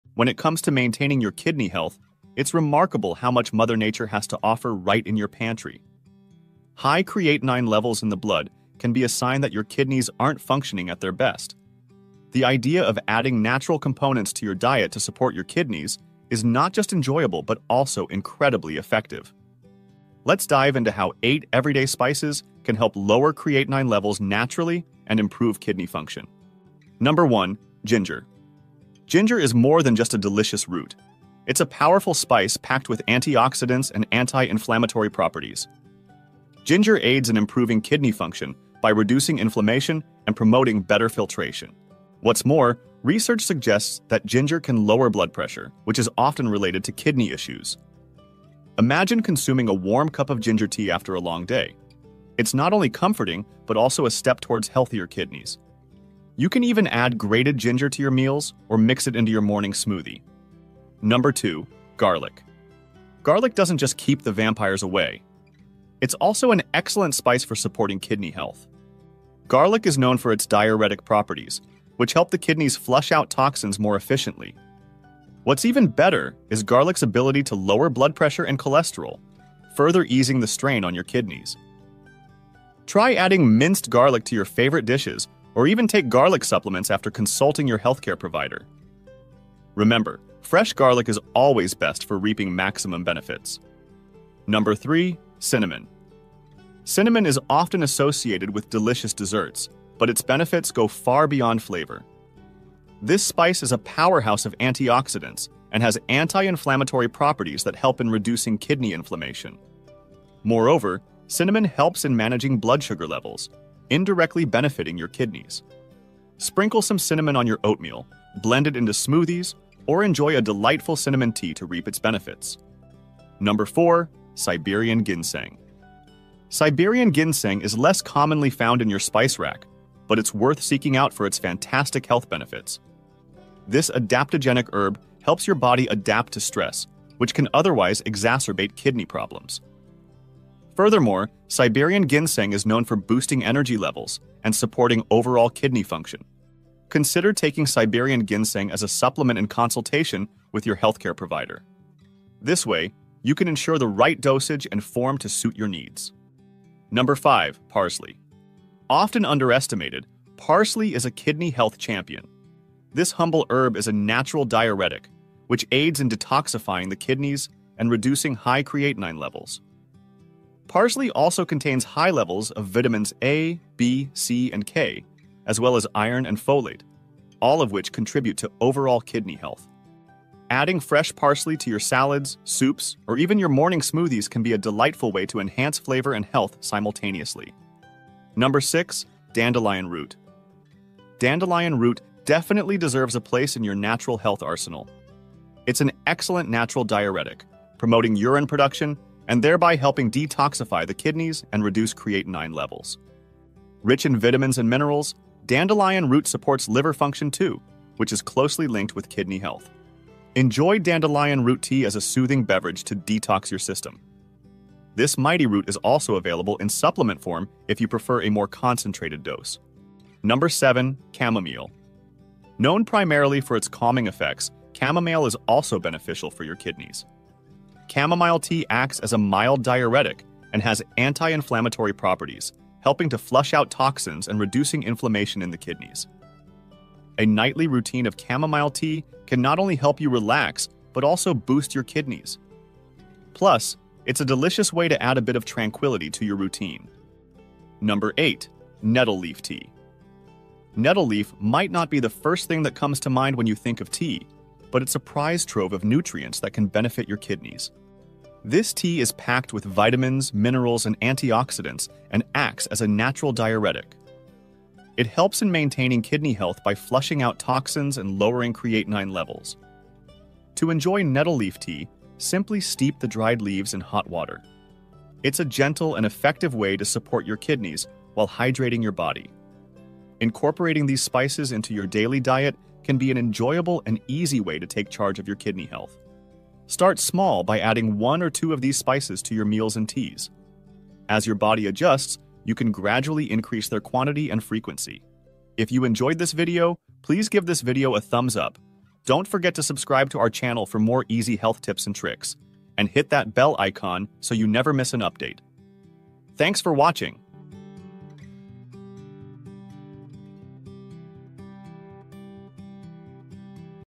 8 Amazing SPICES To Lower Creatinine Levels Naturally - Improve Kidney Function
Hey everyone! Are you looking to improve your kidney function and lower creatinine levels? In this video, we dive into the 8 best spices that can help you do just that! Whether it's 🧄 Garlic, 🍃 Parsley, or soothing ☕ Chamomile, these natural remedies could make a big difference in your health.
1) 🥳 Ginger - Boosts digestion and acts as a natural anti-inflammatory.
2) 🧄 Garlic - Vital for detox and supports your kidneys.
3) 🌾 Cinnamon - Helps manage blood sugar and kidney health.
4) 🌱 Siberian Ginseng - Enhances energy and kidney function.
5) 🍃 Parsley - A powerful diuretic, flushes toxins naturally.
6) 🌼 Dandelion Root - Promotes urine production and healthy kidneys.
7) 🌸 Chamomile - Calm your body and detoxify your kidneys.
8) 🌿 Nettle Leaf Tea - Rich in nutrients, great for kidney support.
⏰Timestamps:
⏰Intro- 00:00
⏰Ginger- 00:27
⏰Garlic- 01:01
⏰Cinnamon- 01:34
⏰Siberian Ginseng- 02:03
⏰Parsley- 02:37
⏰Dandelion Root- 03:08
⏰Chamomile- 03:40
⏰Nettle Leaf Tea- 04:08
Related videos you might like:
Credit for Visuals and Music:
Visual: storyblocks /pexels /pixabay /gettyimages / other